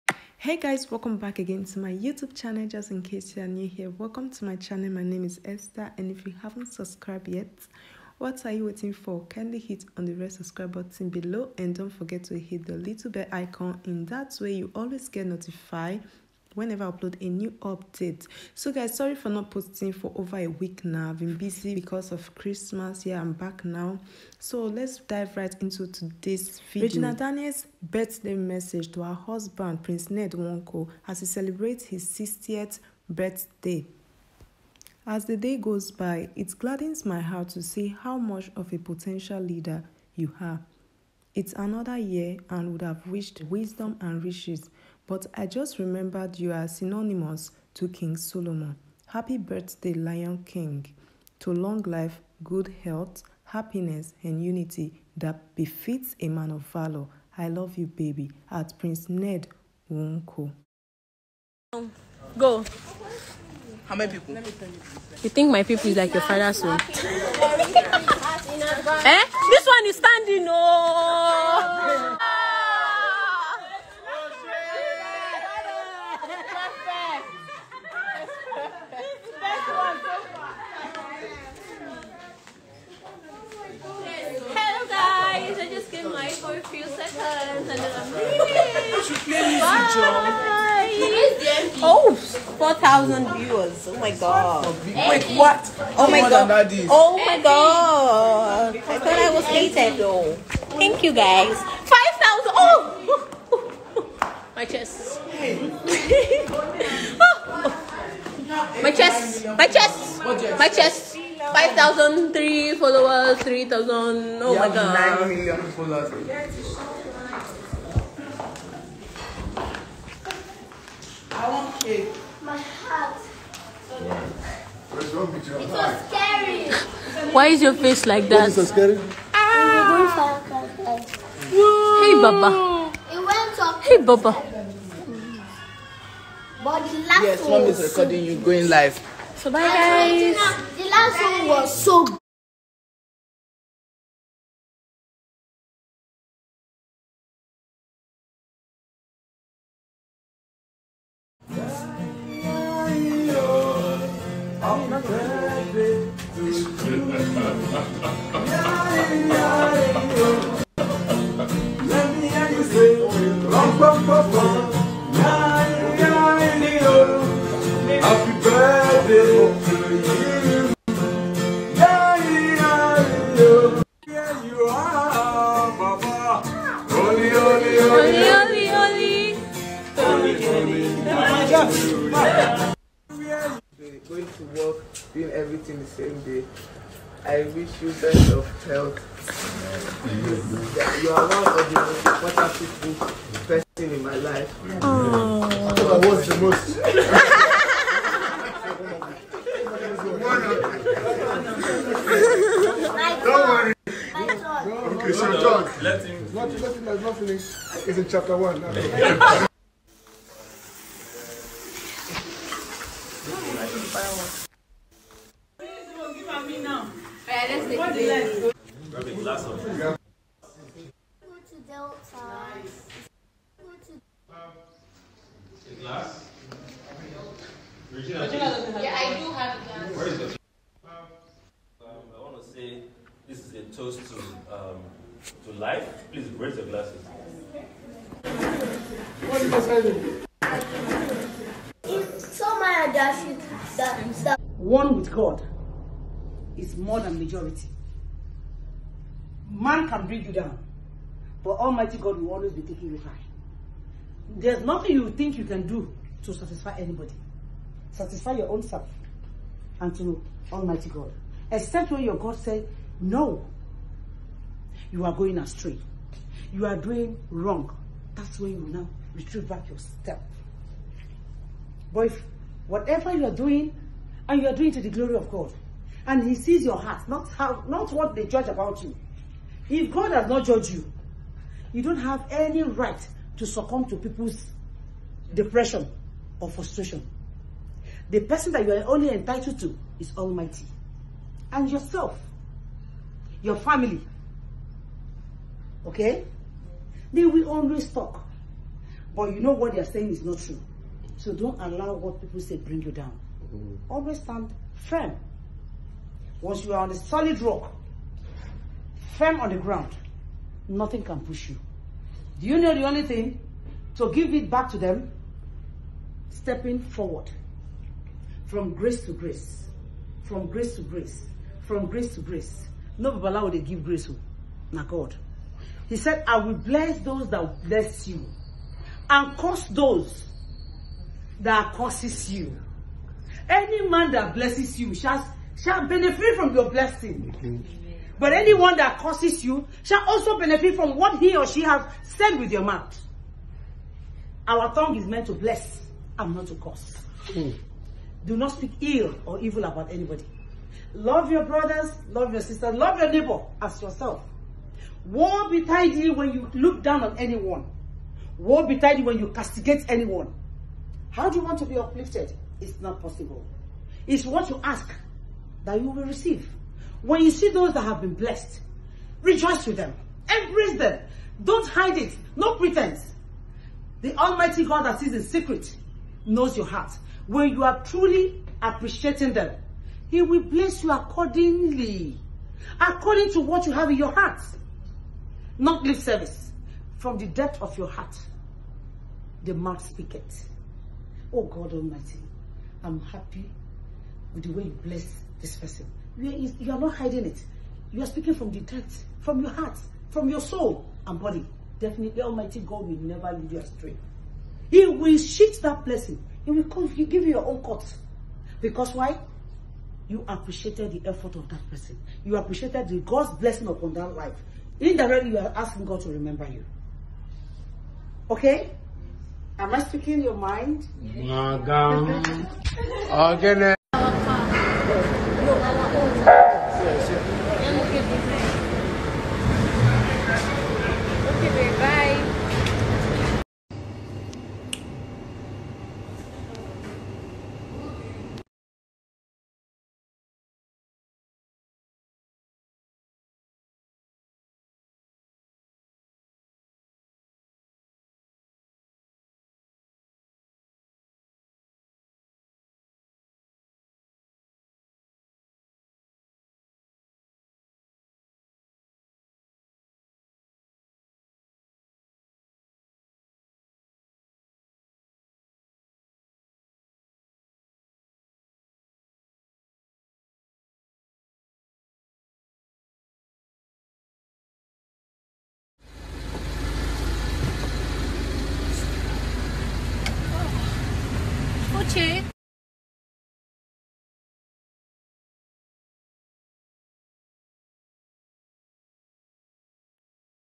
Video How Regina Daniels surprise Ned Nwoko @ his 60th Birthday/ Mama Regina Message to...
Video How Regina Daniels surprise Ned Nwoko @ his 60th Birthday/ Mama Regina Message to...Full video